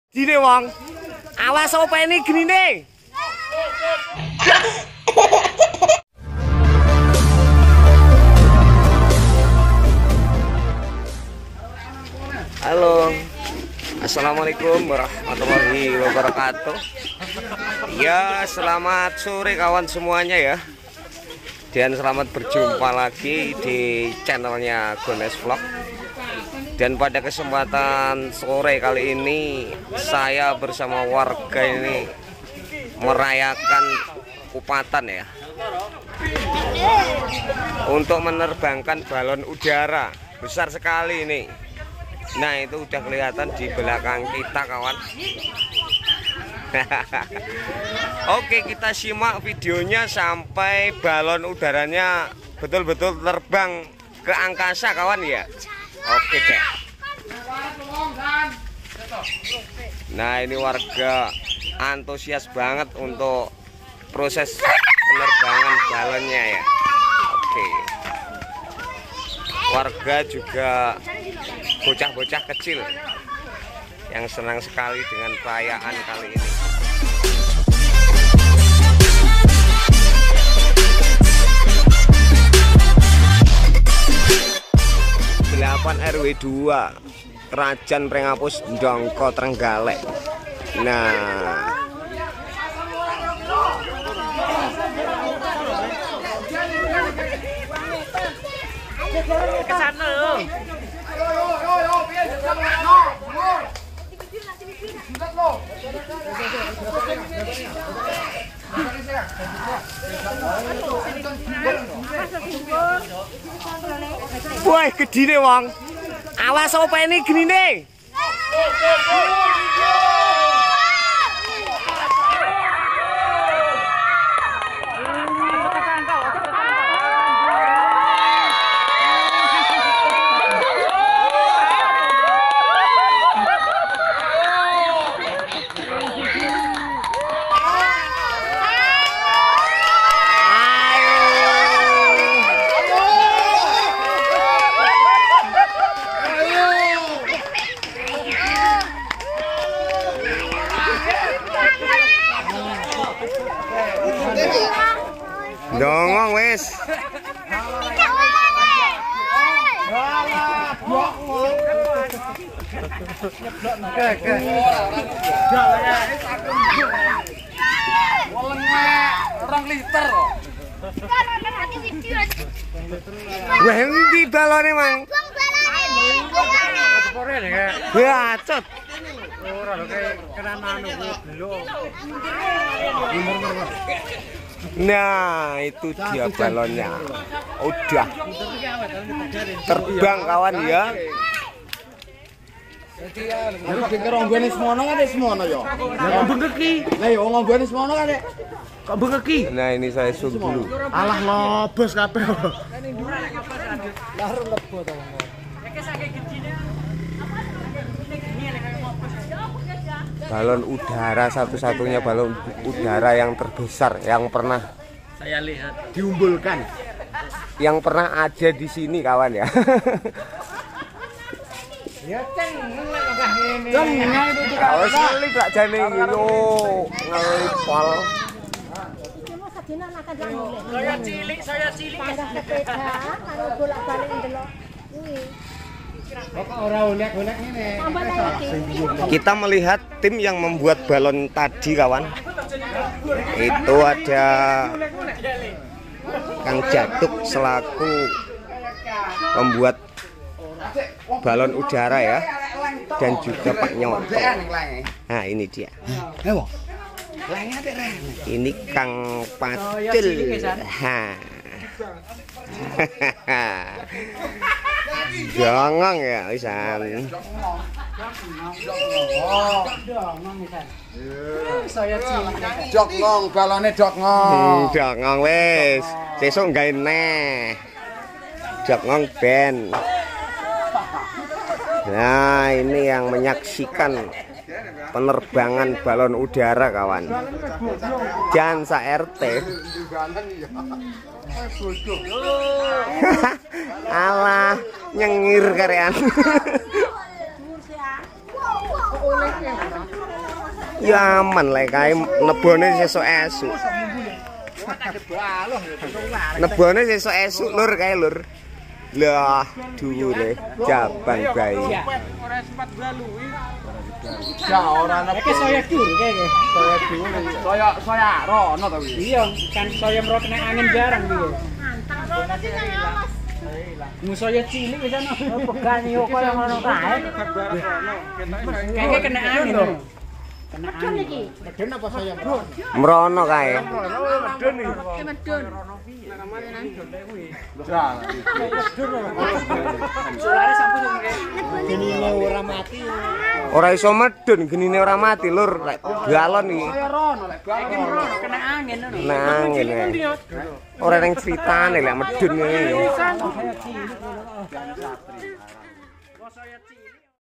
"PENERBANGAN BALON UDARA"
oke pemiesa .di video ini saya mempersembahkan terbangnya balon udara ,
bersama warga kami,bersuka ria merayakan KUPATAN,dengan menerbangkan balon udara.

team perakitan:
1.sdr jadug/kang
2.sdr
3.sdr#tumiran/kang pacul
4.mbah benggoh/coky_andrian
5.sdr kempleng#sunarto
+ dibantu warga.

TERIMAKASIH